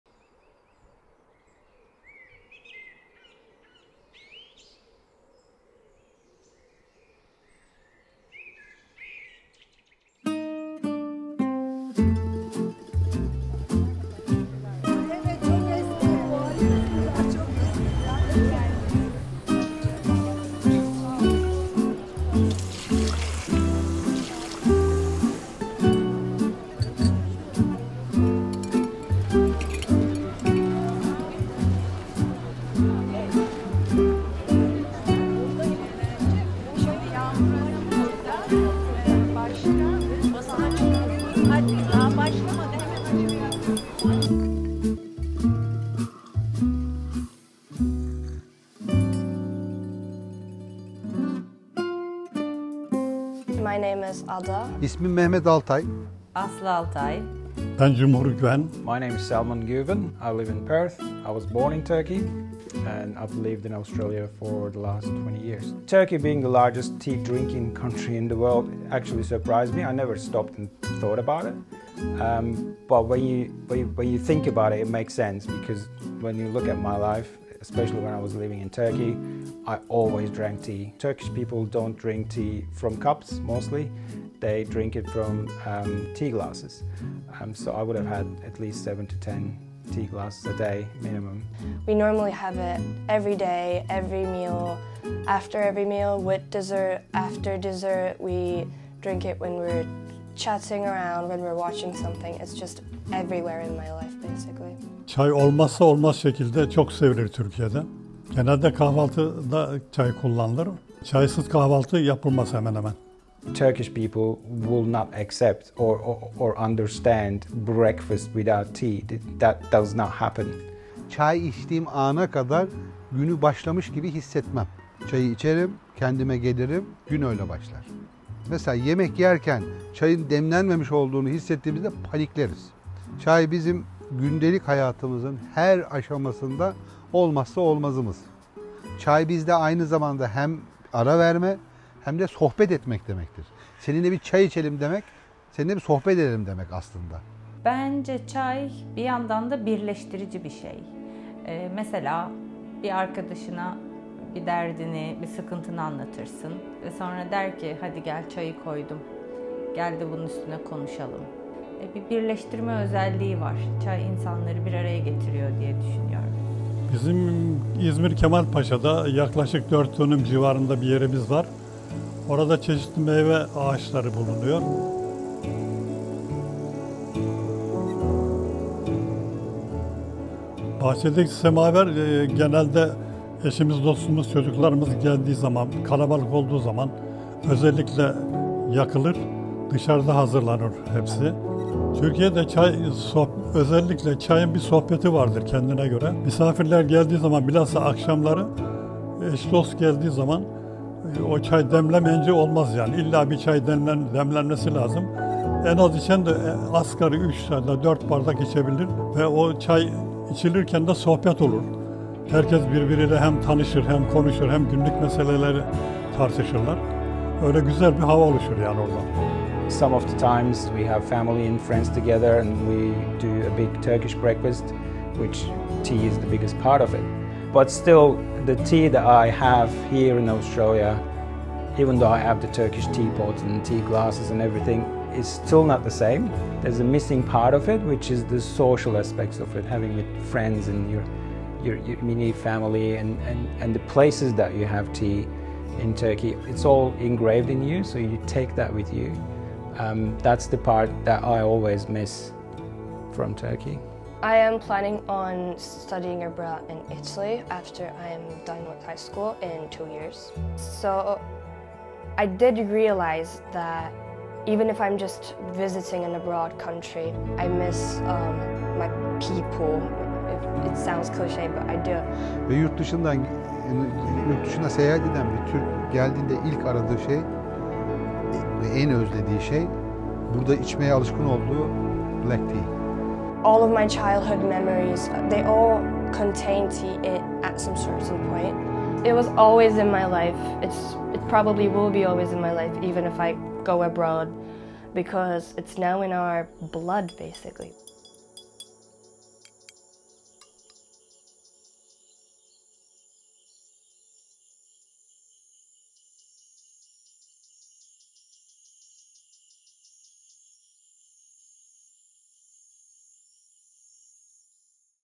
Çay Hazır! (Tea's Ready!) | a short documentary by Enes Guven
A 5-minute documentary which explores how tea culture in Turkey holds a significant role within the lives of Turkish people, always surrounding them and bringing them together.

Directed, edited and filmed by Enes Guven

Starring: Selman Guven, Cumhur Guven, Ada Altay, Aslı Altay, Mehmet Altay